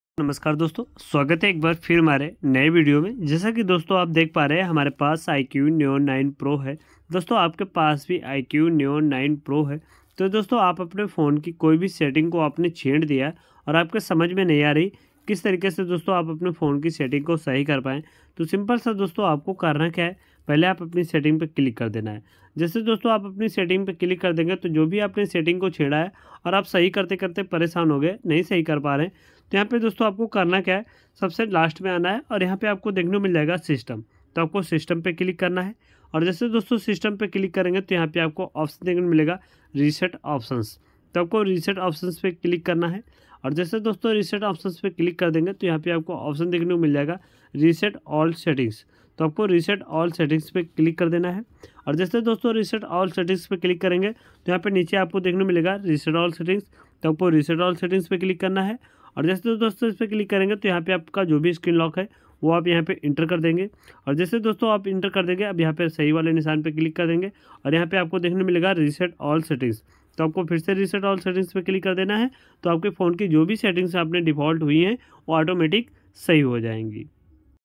iqoo neo 9 pro ki setting sahi kaise kare | iqoo neo 9 pro reset setting
Doston aaj ke is video mein humne aapko bataya ki iqoo neo 9 pro me setting reset kaise karte hain